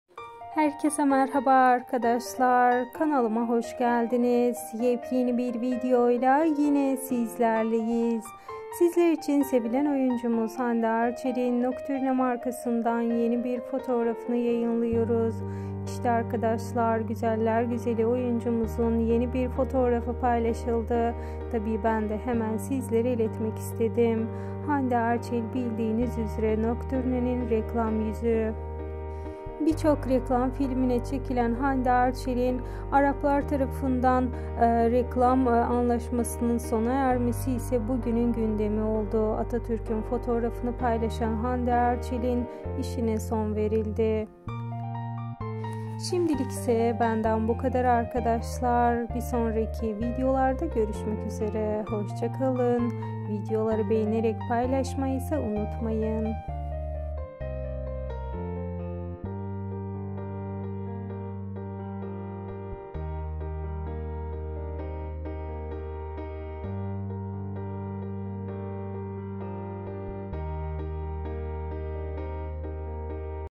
Nocturne Hande Erçelin muhteşem bir Fotosunu paylaştı
Hande Erçel ve Burak Deniz kamera arkası muhteşem kareler geldi